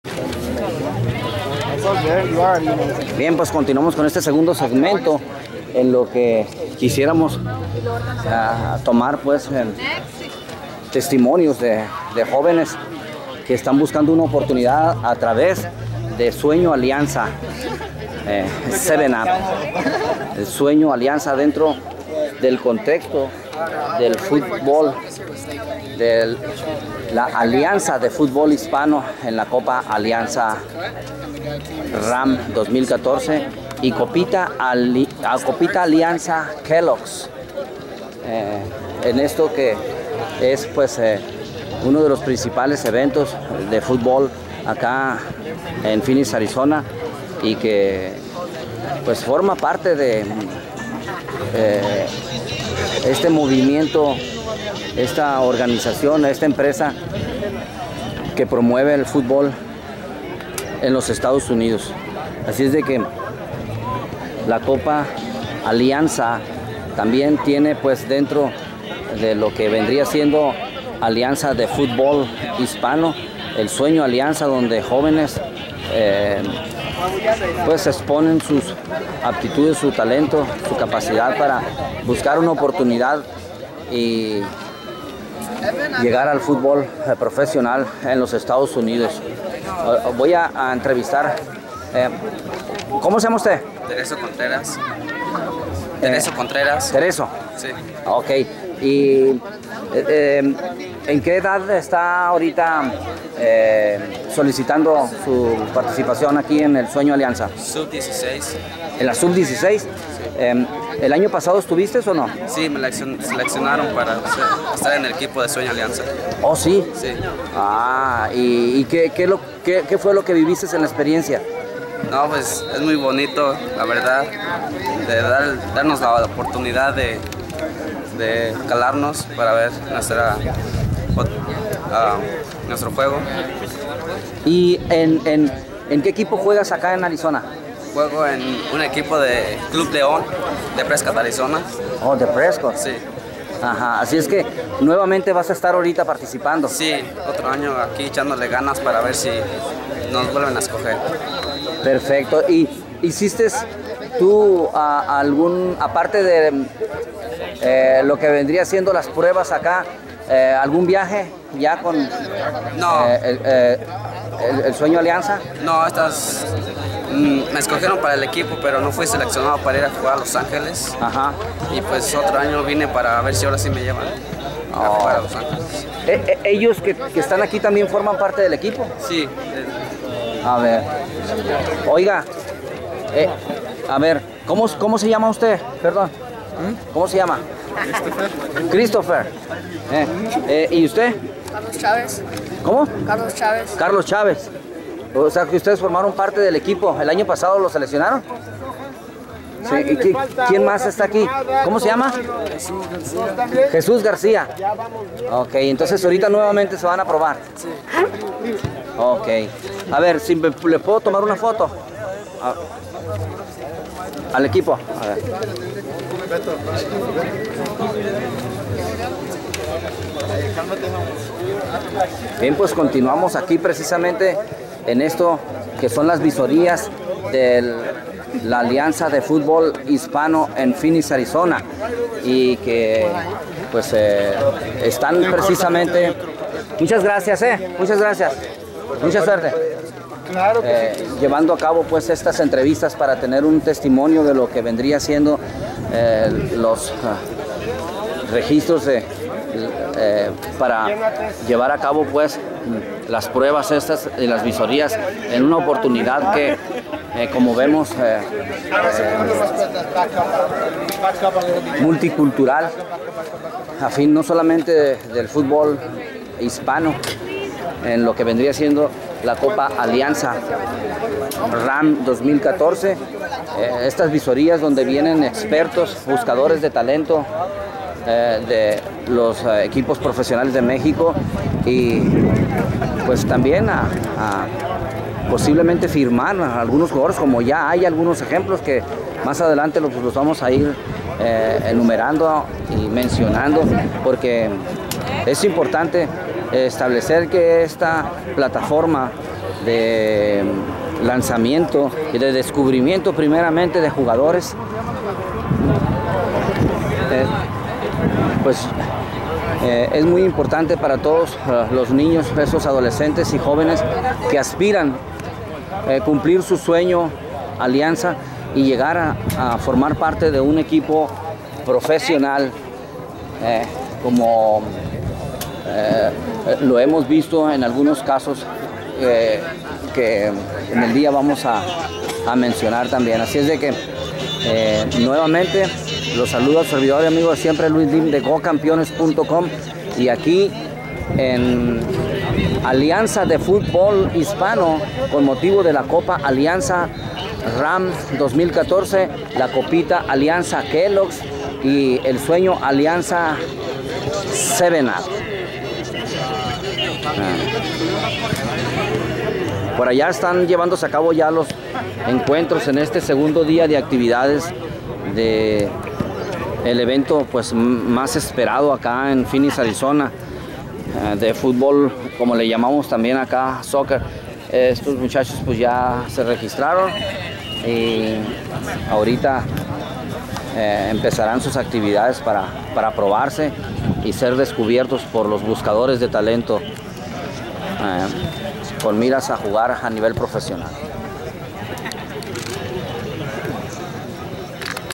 Copa Alianza Ram 2014 Visorias Sueños 7UP SAB 17 Phoenix GoCampeones com 2
Sueño Alianza 7UP de Futbol Hispano en el Reach 11 Sport Complex de Phoenix, Arizona, 16,17 y 18 de Mayo